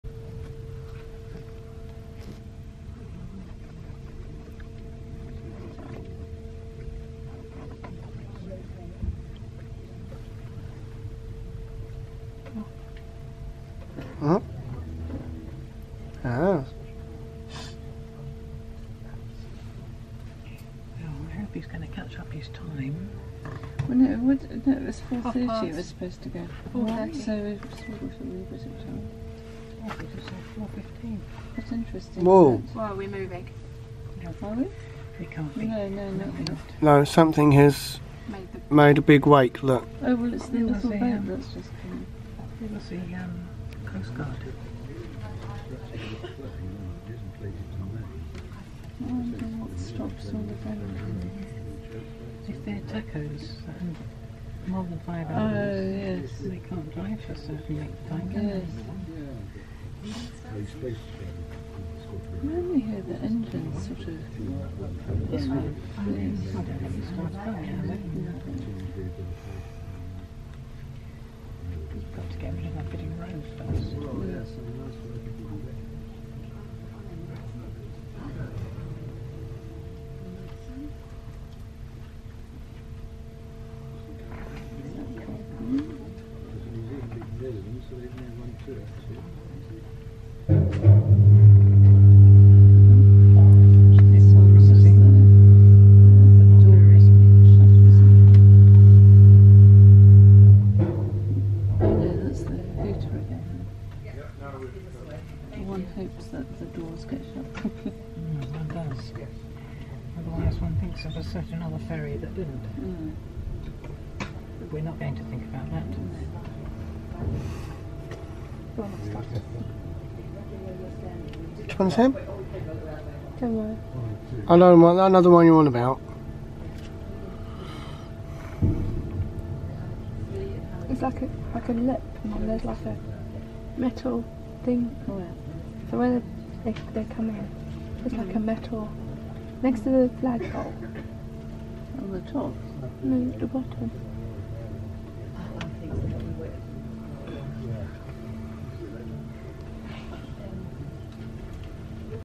Ferry from Caen to Portsmouth Pt2
This is just a video of some more happenings before we actually pulled away from where we were docked.